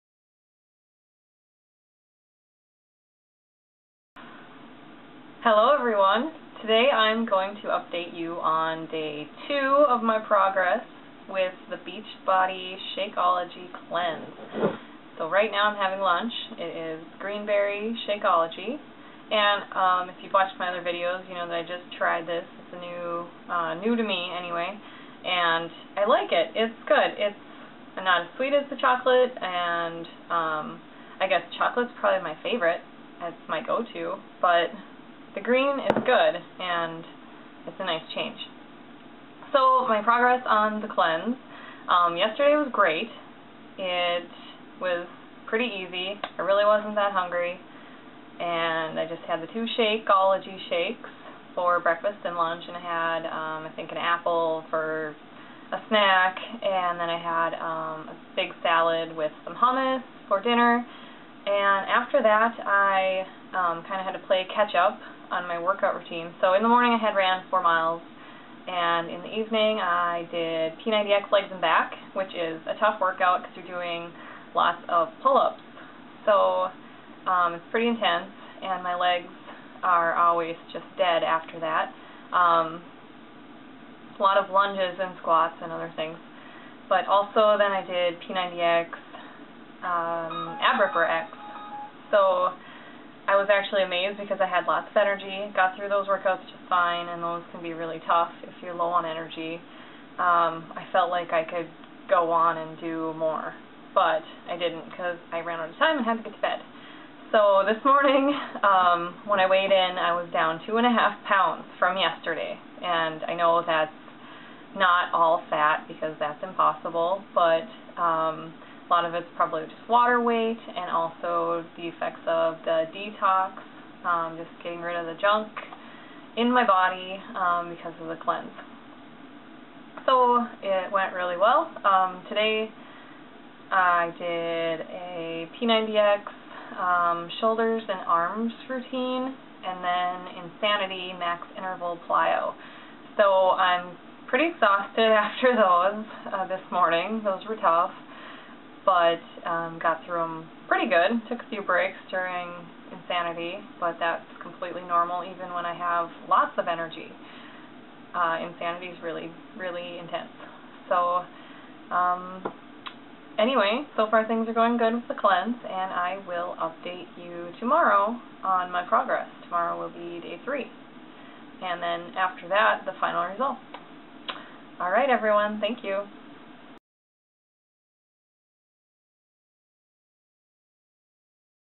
Shakeology Cleanse - Day 2
Day 2 update on the Shakeology Cleanse - 2.5 pounds lost!